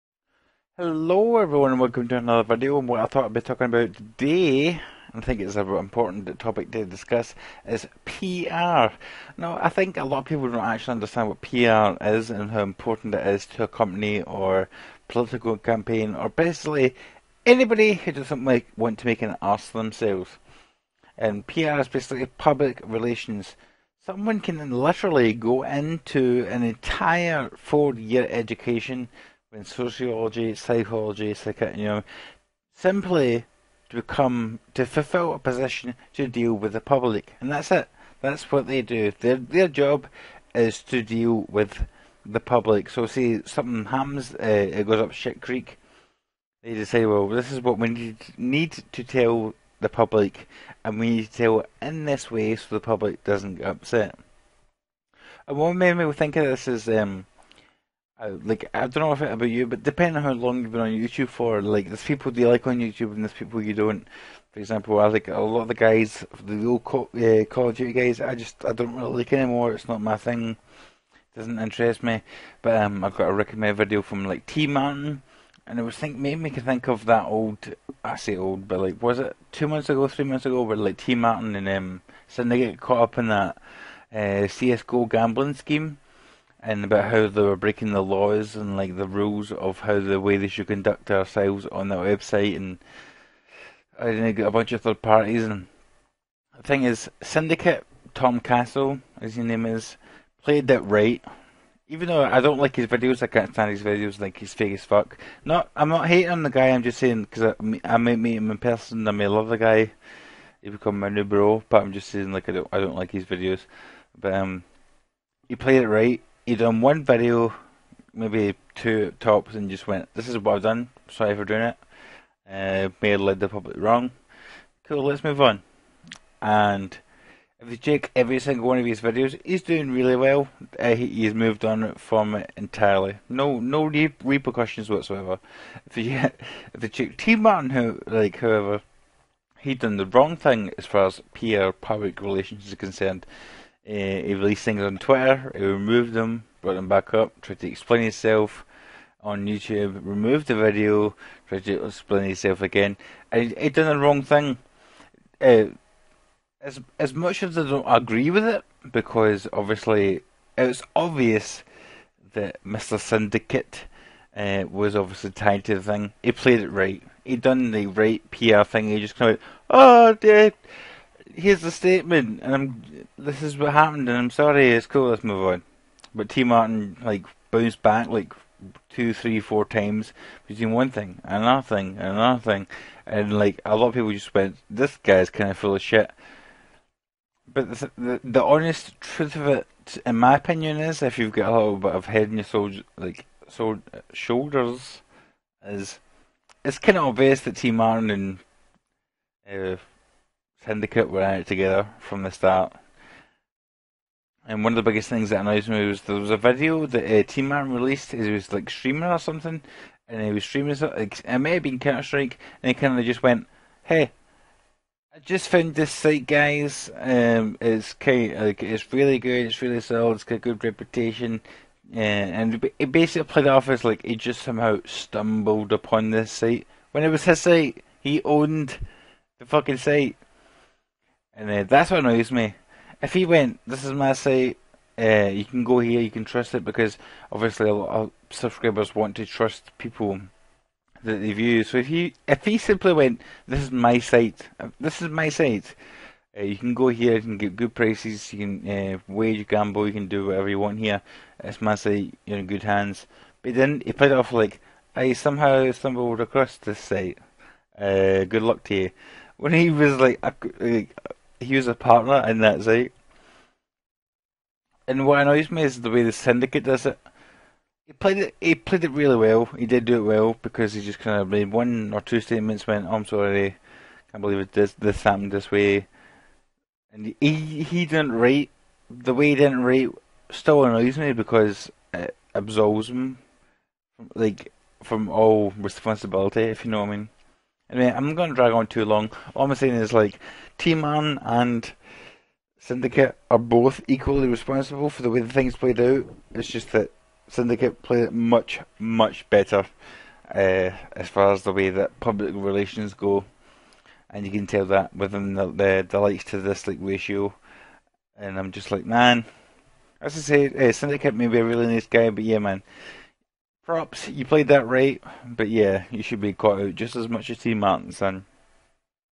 Learn a PR lesson from Tmartn and Syndicate
A crude observation of the lotto scandal they faced a while back,who do you think is paying for it?.A hint, not Syndicate.